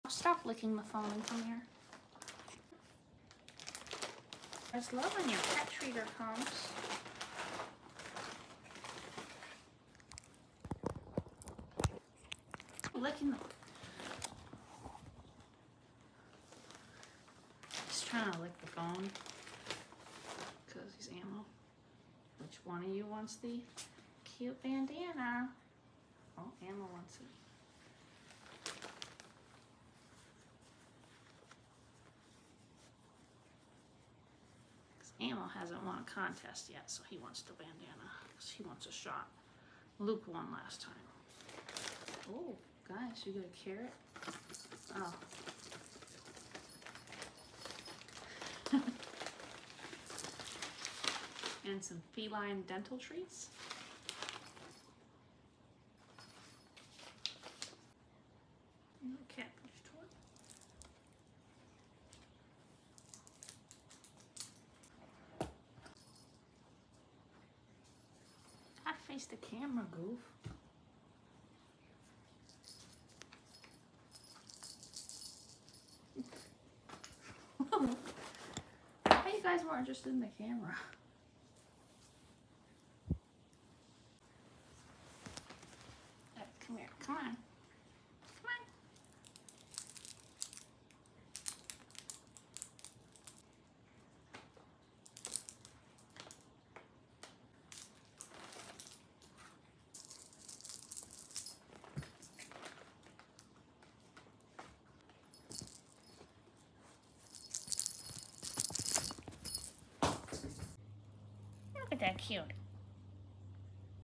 Pet Treater Unboxing with 2 kitty cats! November 2022
Watch these two adorable cats get presents, toys, and treats. Cute kitty cat opening his subscription box / bag! 😻